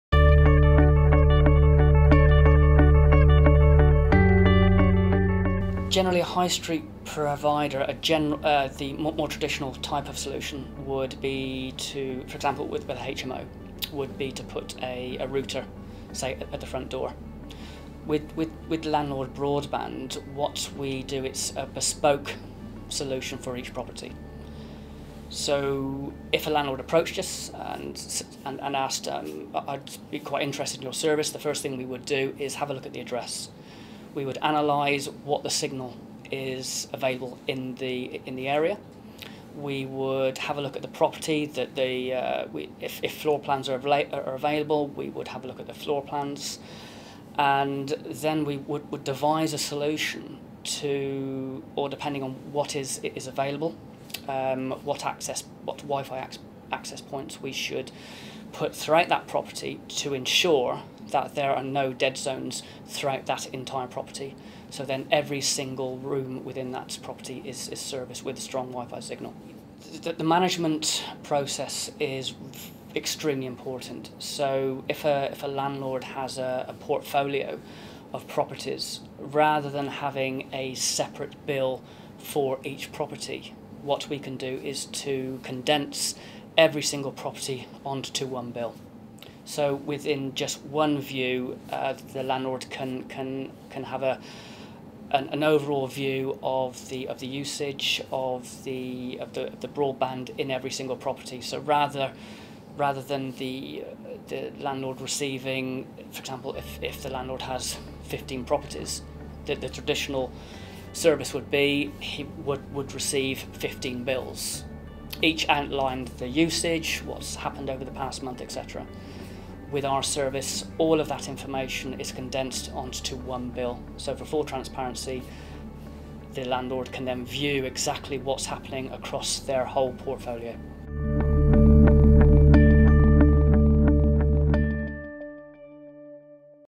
Differences between Landlord Broadband and retail provider
David Spillane of Landlord Broadband explains the important differences between a bespoke installation and wi-fi from one of the big names.